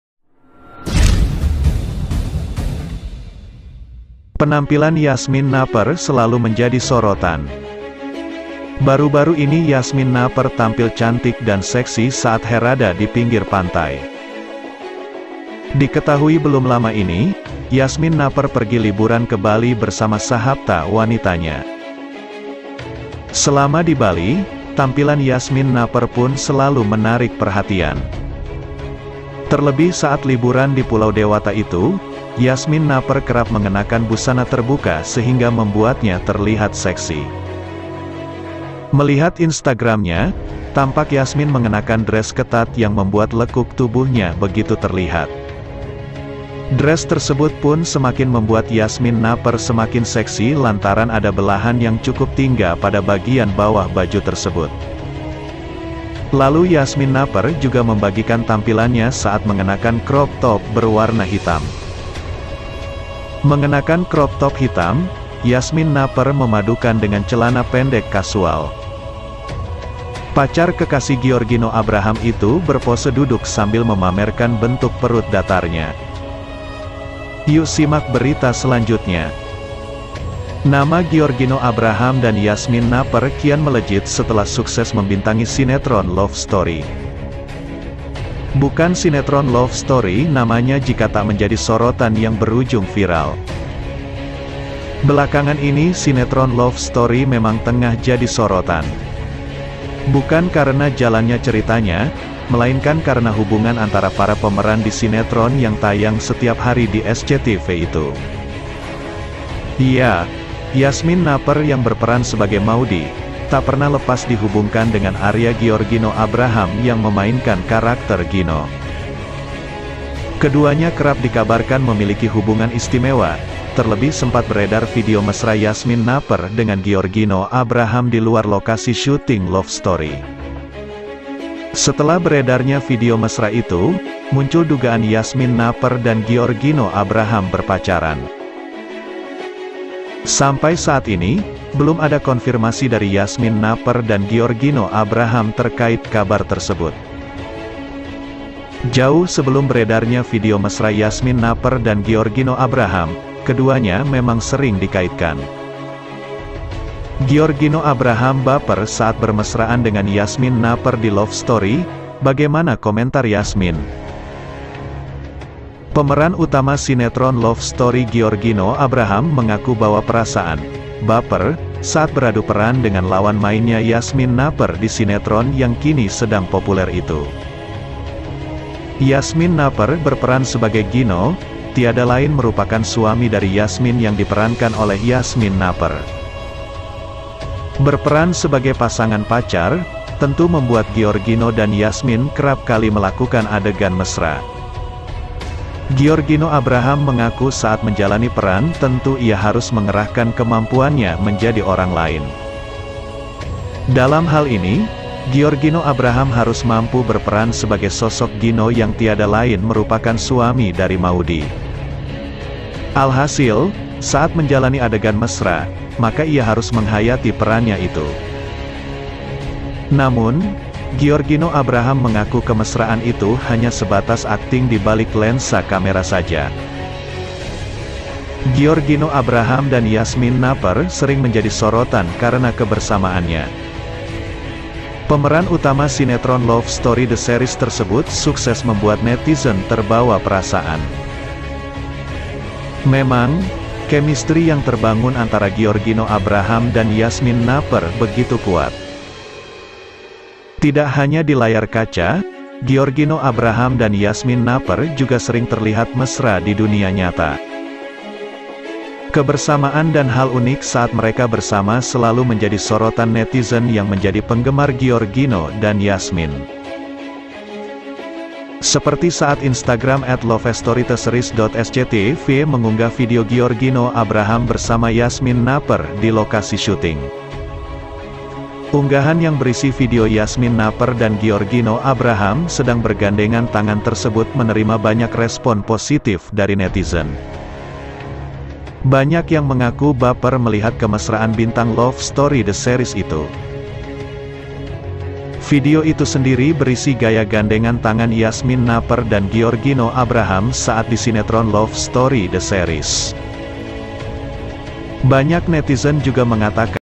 Virall..!! Intip Penampilan Seksi Yasmin Napper Pakai Kemeja Transparan, Bikin Publik Heboh.
Hallo gaiz,ketemu lagi di channel SELFI FAINANI TV.
Kali ini saya akan Memberikan info mengenai Giorgino Abraham dan Yasmin Nepper !!